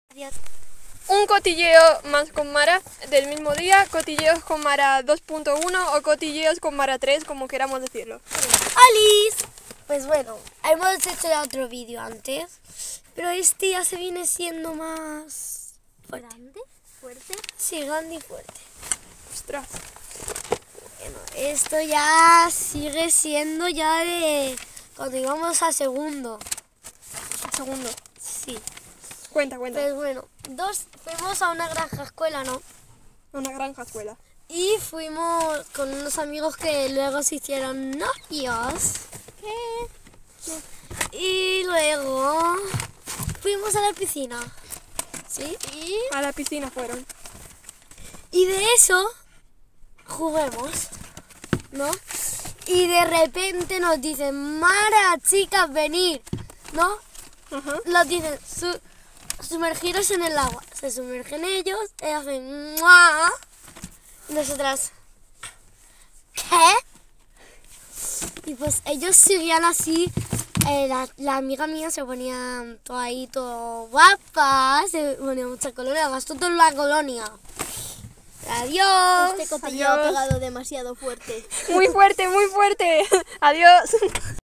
Cotilleos con Mara 3
La historia de la granja escuela. Este viene fuerte!!!

(Es muy largo, así que no lo hemos podido convertir en short)